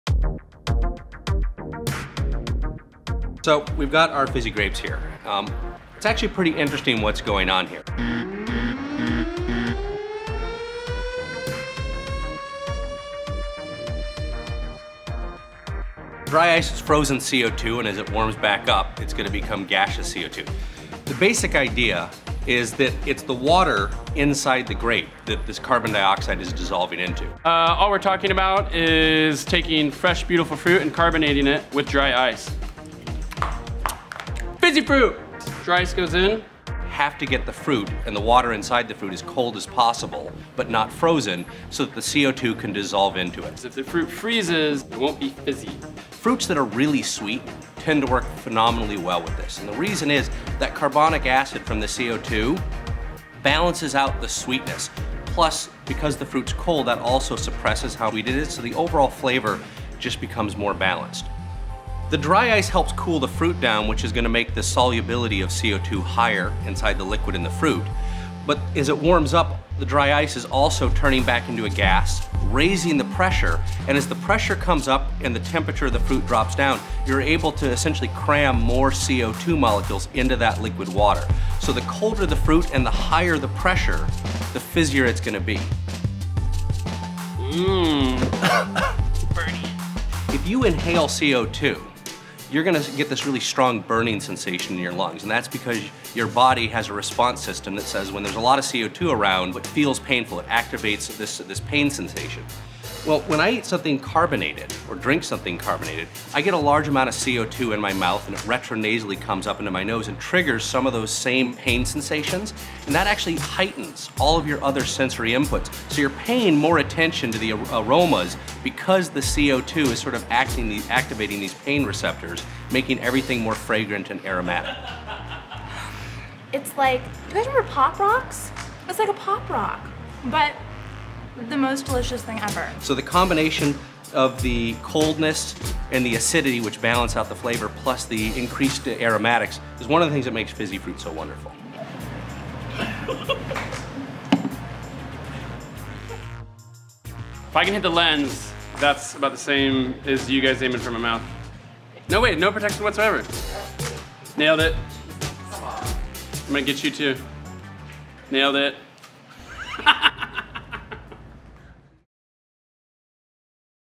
ChefSteps Nerd Alert: The Science of Fizzy Fruit
Greetings fellow nerd friends! Want to learn the science of carbonating fruit with dry ice? Right this way